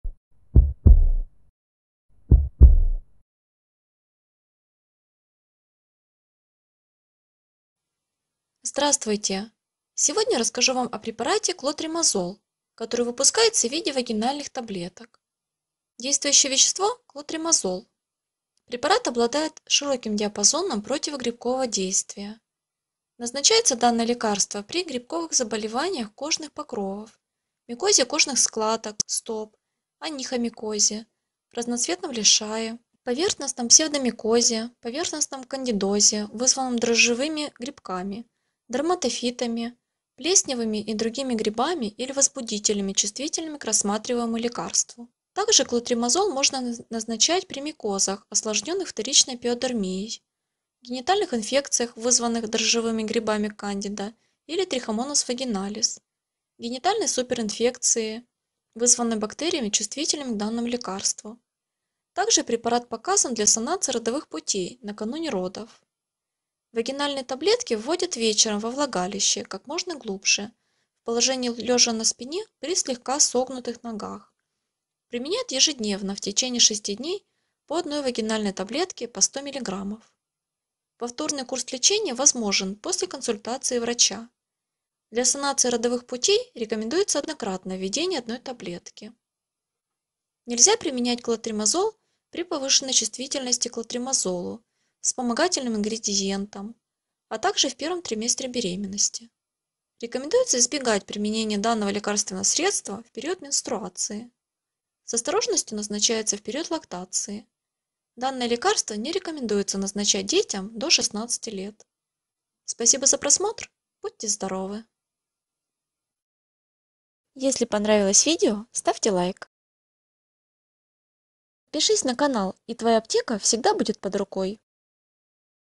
Клотримазол таблетки - показания (видео инструкция) описание, отзывы
Клотримазол – противогрибковое лекарство. Обладает широким диапазоном противогрибкового действия. Действующее вещество препарата – имидазольное антимикотическое средство. Его противогрибковый эффект обусловлен способностью действующих молекул нарушать состав мембраны клеток грибов, изменяя ee проницаемость, что приводит к лизису (распаду) клеток возбудителя. Данное лекарство эффективно против дерматофитов, a также дрожжевых грибов. Кроме того, препарат имеет противомикробное действие – активно против грамположительных (многие стафилококки, стрептококки), грамотрицательных бактерий (Gardnerella vaginalis, Bacteroides spp.), a также Trichomonas vaginalis, Corynebacterium minutissimum, Malassezia furfur.

При интравагинальном применении (введении во влагалище) активное вещество действует местно – в кровоток попадает всего 5–10% лекарства. Абсорбировавшееся (всосавшееся) вещество метаболизируется в печени, выводится с желчью.

Фунгицидная концентрация препарата сохраняется во влагалище на протяжении трех дней после нанесения.

Назначается данное лекарство при грибковых заболеваниях кожных покровов, микозе кожных складок, микозе стоп, онихомикозе (грибке ногтей), разноцветном лишае, поверхностном псевдомикозе (эритразме), поверхностном кандидозе, вызванном дрожжевыми (в т.ч. Сandida), дерматофитами, плесневыми, другими грибами или возбудителями, чувствительными к рассматриваемому лекарству.

Также Клотримазол можно назначать при микозах, осложненных вторичной пиодермией (гнойным поражением кожи), генитальных инфекциях, вызванных дрожжевыми грибами Сandida или Trichomonas vaginalis (трихомониазе, кандидозном вульвовагините), генитальной суперинфекции (присоединении еще одного вида инфекции), вызванной бактериями, чувствительными к данному лекарству.

Также показан для санации родовых путей накануне родов.

Противопоказания
Нельзя применять Клотримазол при повышенной чувствительности (аллергии) к клотримазолу, вспомогательным ингредиентам, a также в I-м триместре беременности.

Рекомендуется избегать применения данного лекарственного средства в период менструации.

С осторожностью назначается в период лактации.

Данное лекарство не рекомендуется назначать детям до 16 лет.

При проведении клинисследований не было установлено негативного влияния данного лекарства на течение беременности, возникновение осложнений в родах, развитие плода. Однако вопрос о целесообразности назначения лекарства для беременных пациенток или кормящих матерей должен решать доктор индивидуально.

В период беременности введение вагинальных таблеток нужно проводить без аппликатора.

Действующее вещество:
Клотримазол (Clotrimazolum)

Назначается при:
Вагинальный кандидоз
Дерматит
Красный плоский лишай
Лишай
Стригущий лишай

Код АТХ:
D01AC01 - Клотримазол
G01AF02 - Клотримазол

Бетазон
Имазол
Кандибене
Кандид
Кандидерм
Канестен
Триакутан
Тридерм
Фунгисан

Внимание!